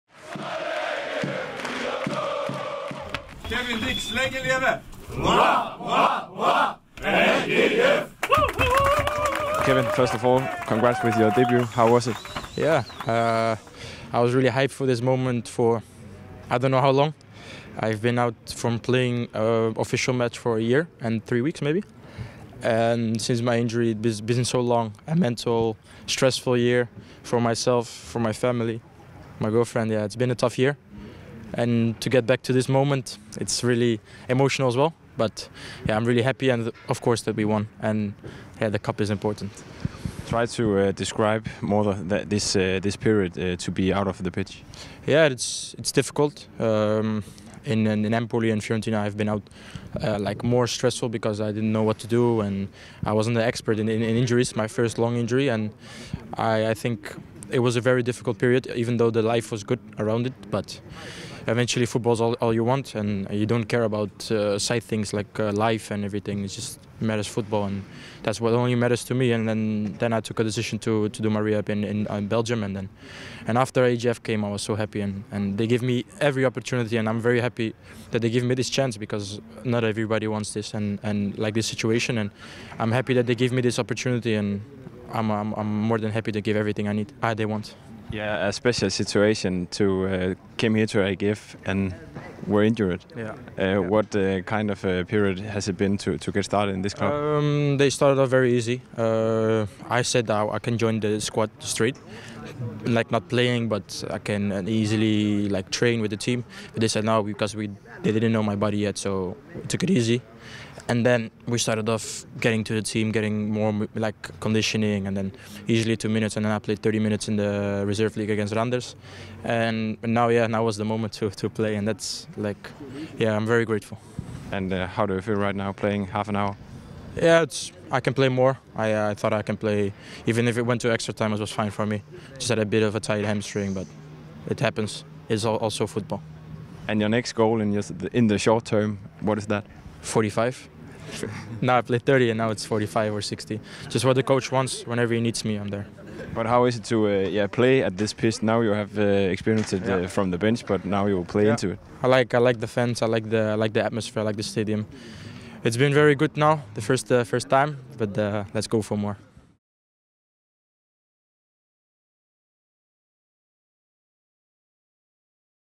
Debut til Diks
Torsdag aften fik Kevin Diks debut i den hvide trøje efter han kom til klubben i sommer og først skulle komme sig fra en skade. Så med en sejr oven i hatten var det en glad hollænder som AGF TV mødte efter kampen.